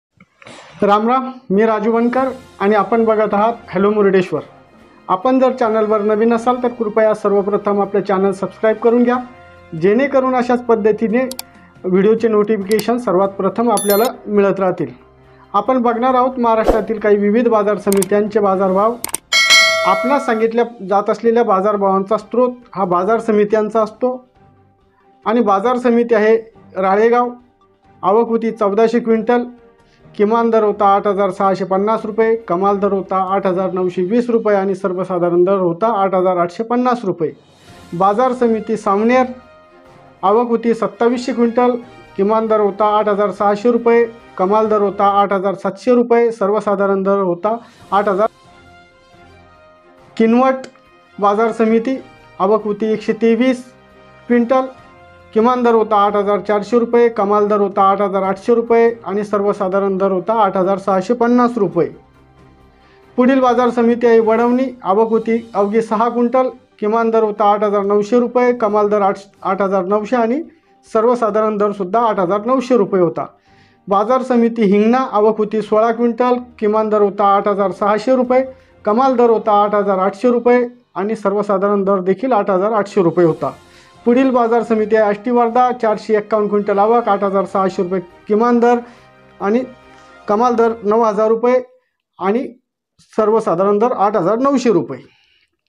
हे आहेत महाराष्ट्रातील कापसाचे बाजार भाव/kapus bajar bhav
नमस्कार शेतकरी मंडळी मी राजु बनकर आपल्या हॅलो मुर्डेश्वर या चॅनलमध्ये मनापासून स्वागत करतो सध्या सर्वत्र चर्चेचा आणि आपल्या आस्थेचा विषय असलेल्या कापसाचे बाजार भाव सांगताना आम्हाला अत्यानंद होत आहे महाराष्ट्रातील काही महत्त्वाच्या आणि ऐनवेळी उपलब्ध होणारे कापूस बाजार भाव दररोज पोहोचतील आपण मिरची बाजार आणि बैल बाजार या विषयांना जे प्रेम दिले तेच प्रेम आपण कापूस बाजार भाव या विषयाला अशी आशा बाळगतो